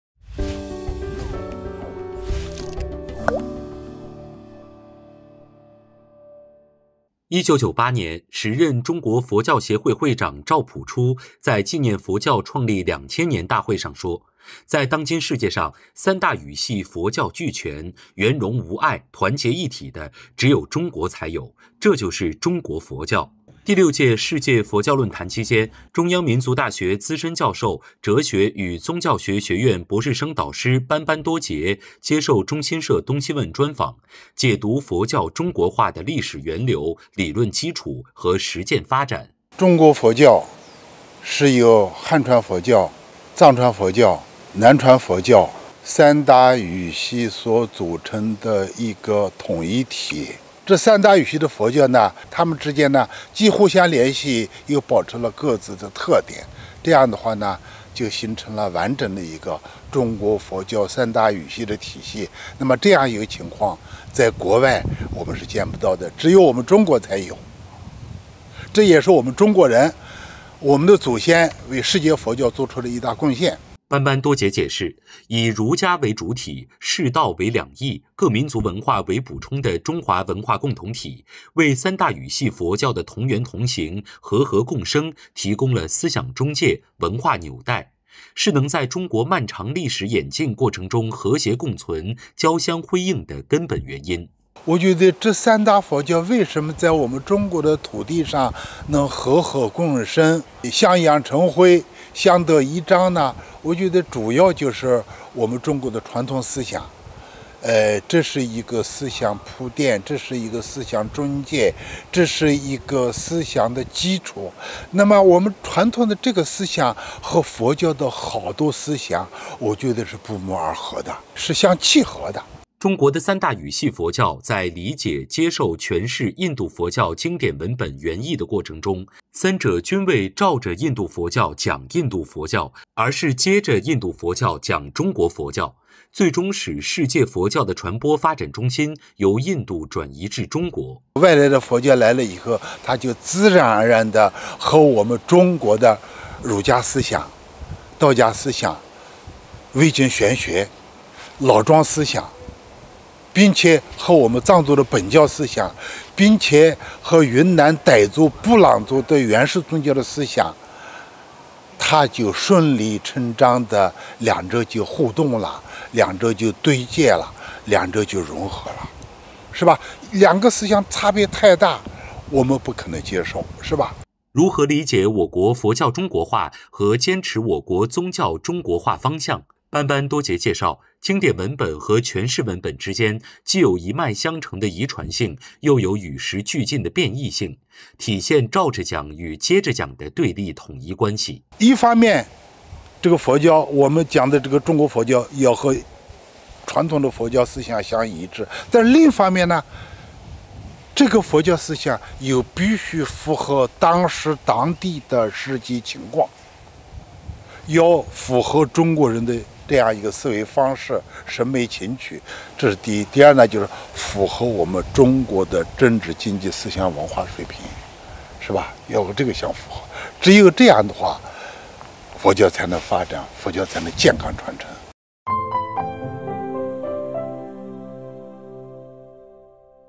【东西问】班班多杰：三大语系佛教为何能在中国交相辉映？
同源同行，和合共生，中国佛教是一个大家庭，三大语系佛教流传时间有先后之别，但均为中国佛教组成部分。1998年，时任中国佛教协会会长赵朴初在纪念佛教创立两千年大会上说，在当今世界上，三大语系佛教俱全，圆融无碍，团结一体的，只有中国才有，这就是“中国佛教”。

中国三大语系佛教从何而来？各自有何特征？为什么能在中国和谐共存、交相辉映？中央民族大学资深教授、哲学与宗教学学院博士生导师班班多杰，近日接受中新社“东西问”专访，解读我国佛教中国化的历史源流、理论基础和实践发展。（让宝奎 崔白露 刘超）